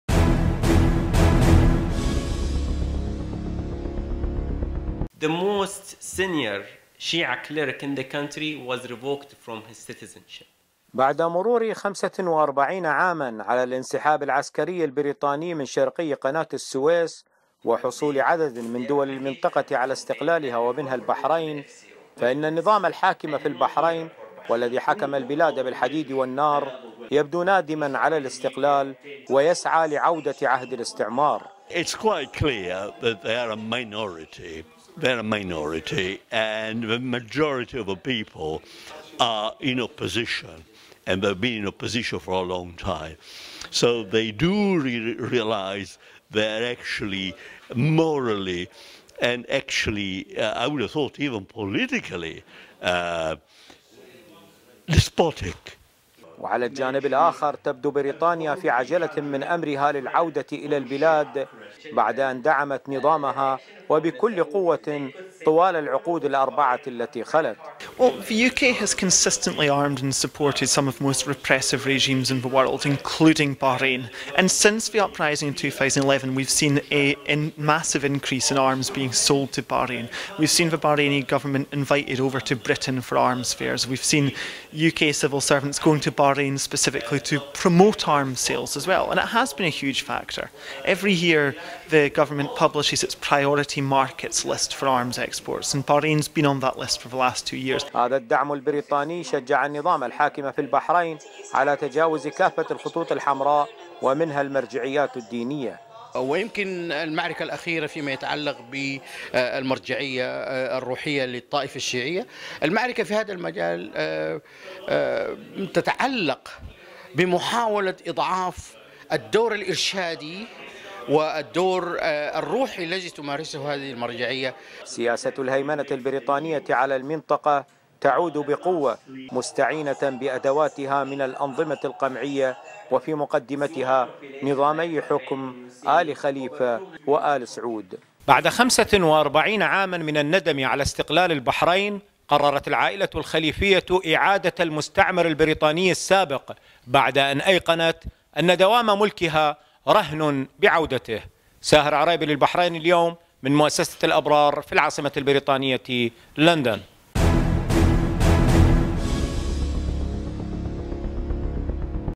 تقرير متلفز عودة بريطانية إلى البحرين بعد ٤٥ عاما من الاستقلال
بعد مرور ٤٥ عاما على الإنسحاب العسكري البريطاني من شرقي قناة السويس وحصول عدد من دول المنطقة على استقلالها ومنها البحرين, فإن النظام الحاكم في البحرين والذي حكم البلاد بالحديد, يبدو نادما على الإستقلال ويسعى لعودة عهد الإستعمار.

وعلى الجانب الآخر تبدو بريطانيا في عجلة من امرها للعودة الى البحرين بعد دعمت نظامها وبكل قوة طوال العقود الأربعة التي خلت.

هذا الدعم البريطاني شجّع النظام الحاكم في البحرين على تجاوز الخطوط الحمراء ومنها المرجعيات الدينية.

سياسة الهيمنة البريطانية على المنطقة تعود بقوة مستعينة بادواتها من الأنظمة القمعية وفي مقدمتها نظامي حكم آل خليفة وآل سعود.